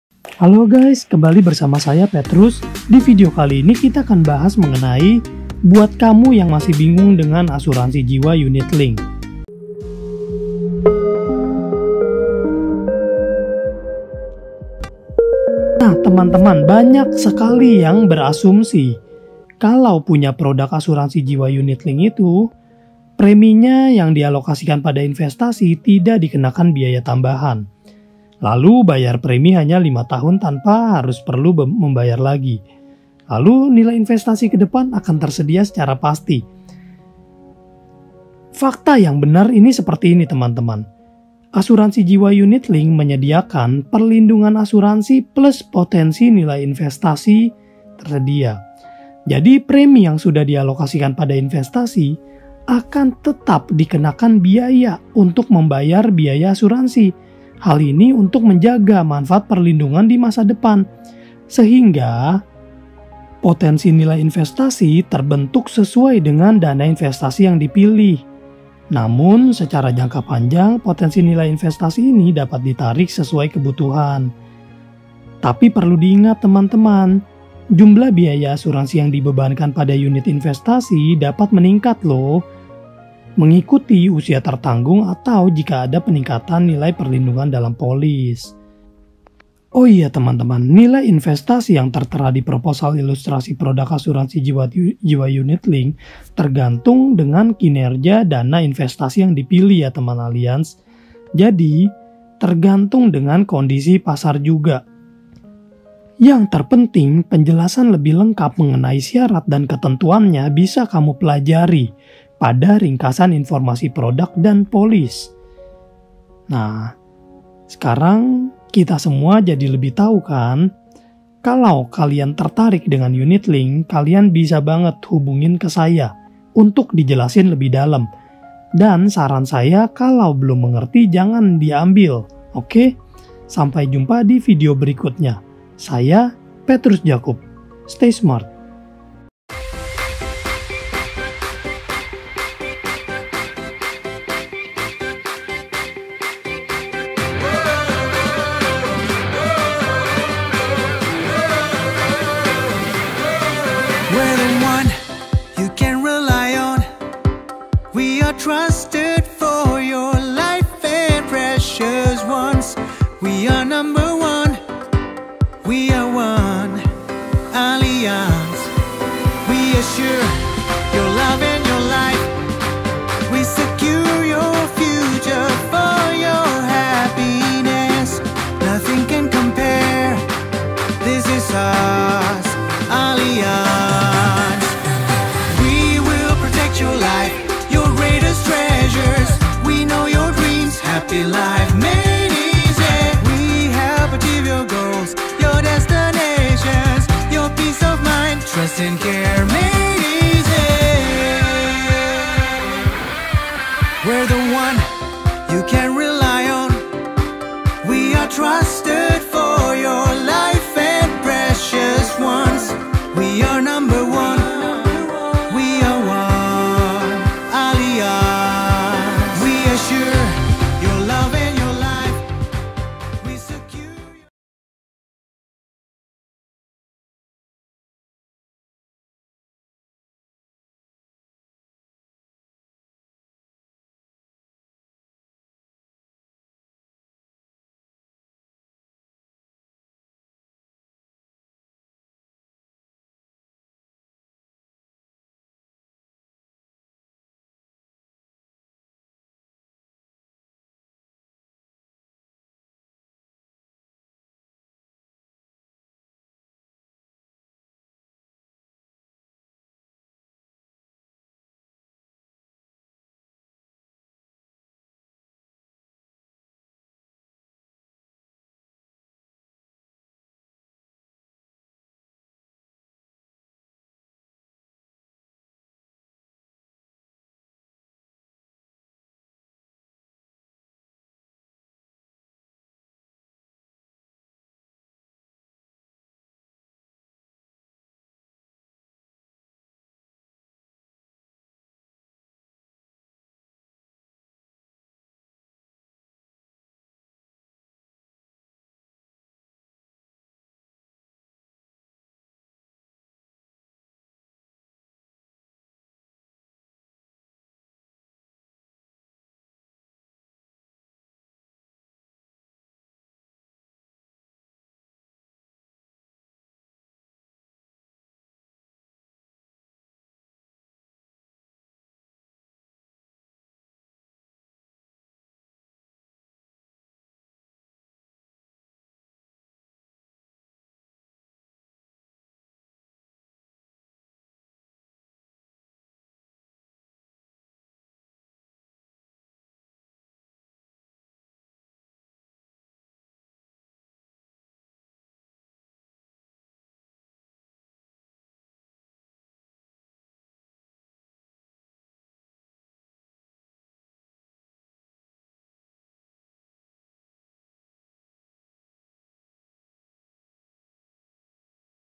Buat Kamu Yang Masih Bingung Dengan Asuransi Jiwa Unit Link
Banyak yang berasumsi kalau punya produk asuransi jiwa unit link itu :

Premi yang dialokasikan pada investasi tidak dikenakan biaya tambahan
Bayar Premi hanya selama 5 tahun tanpa perlu membayar lagi
Nilai investasi kedepannya akan tersedia secara pasti
Fakta yang benar adalah :

Karena asuransi jiwa unit link menyediakan manfaat: PERLINDUNGAN ASURANSI + POTENSI NILAI INVESTASI TERSEDIA

Jadi, Premi yang sudah dialokasikan pada investasi akan tetap dikenakan biaya untuk membayar biaya asuransi. Hal ini untuk menjaga manfaat perlindungan di masa depan.

Sehingga Potensi Nilai Investasi terbentuk sesuai dengan dana investasi yang dipilih. Namun secara jangka panjang, potensi Nilai INvestasi ini dapat ditarik sesuai kebutuhan.

PERLU DIINGAT….

Jumlah biaya asuransi yang dibebankan pada unit investasi dapat meningkat loh. Mengikuti usia tertanggung atau jika ada pengingkatan nilai perlindungan dalam polis.

Oh iya, Nilai Investasi yang tertera di proposal ilustrasi produk asuransi jiwa unit link itu tergantung dengan kinerja dana investasi yang dipilih ya teman Allianz. Jadi tergantung dengan kondisi pasar juga loh.

YANG TERPENTING…

Penjelasan lebih lengkap mengenai syarat dan ketentuannya bisa kamu pelajari pada Ringkasan Informasi Produk dan Polis.

Mari kita berasuransi.

Untuk info lebih lanjut, silakan hubungi kami :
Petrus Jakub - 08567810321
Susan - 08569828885